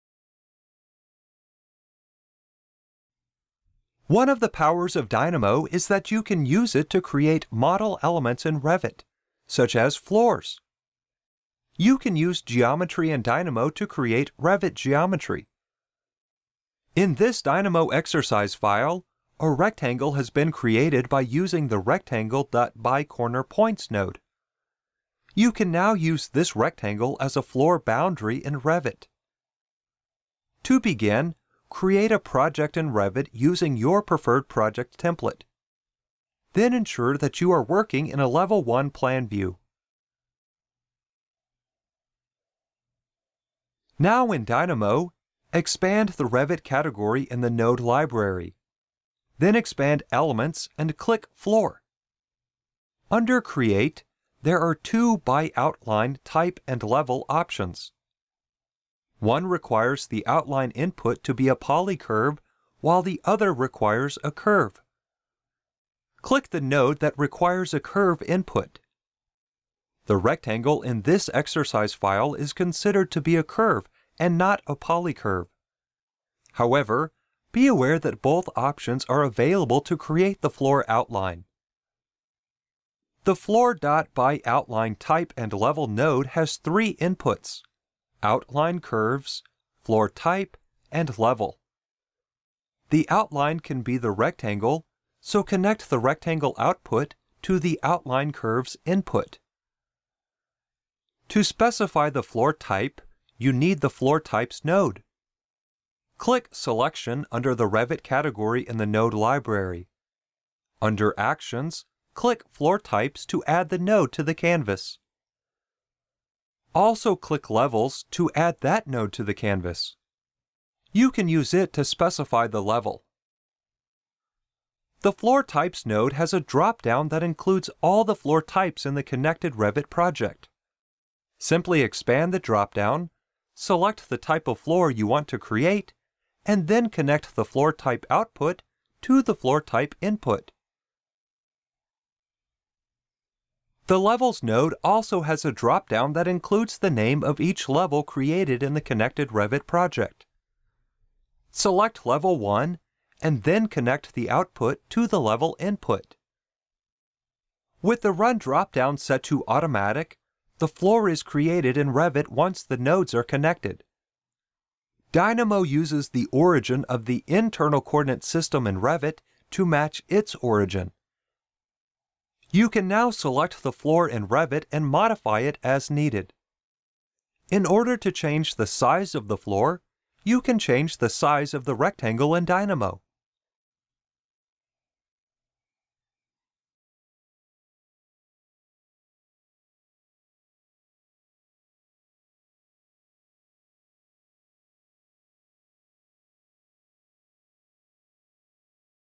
Creating Floors with Dynamo
Learn how to create floors using the visual-programming